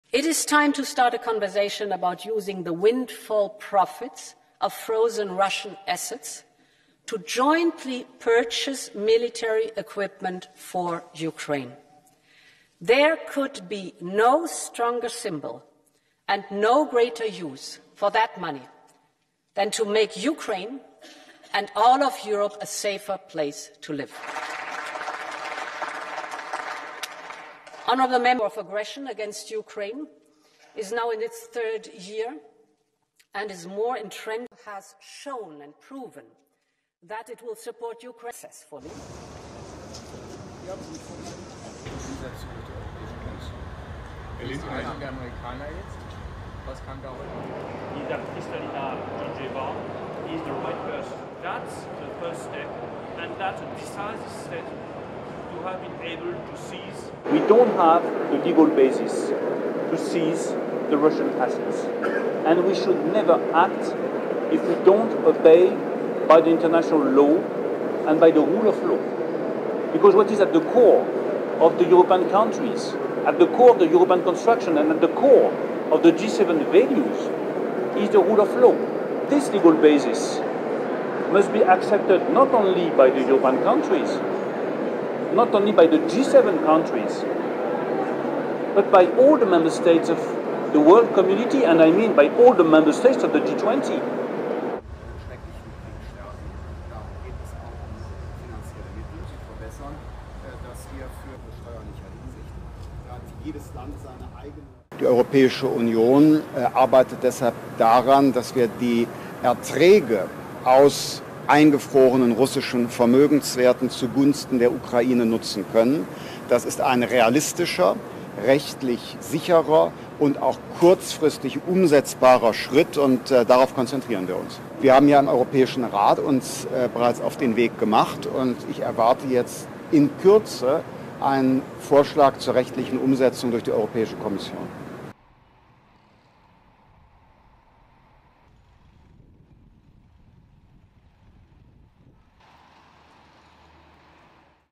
G7 finance meeting marred by divisions over seizing Russian assets
European leaders should discuss using the profits from frozen Russian assets to boost Ukraine’s military in its defense efforts against Moscow, European Commission President Ursula von der Leyen said Wednesday.

Von der Leyen’s comments come after U.S. Treasury Secretary Janet Yellen on Tuesday said it is important to find a way to unlock the value of frozen assets to bolster Ukraine, stressing that there is a “strong international law, economic and moral case” to do so and that it was crucial for allies to work together on the matter.

The European Union has been considering using profits from Russian frozen assets to support Ukraine for some time now. This has included military support as well as helping Ukraine with its reconstruction efforts. However, officials have expressed concerns about both the legality of using frozen assets, as well as potential fallout for global currency markets.

French Finance Minister Bruno Le Maire argued on Wednesday (February 28) that there is not enough basis in international law to seize the Russian assets, emphasizing that such a move would require the endorsement of G20 members and other countries.

And German Finance Minister Christian Lindner said on Wednesday the Group of 20 finance ministers are discussing financing for Ukraine and want to raise the pressure on Russia.

Editor: Zou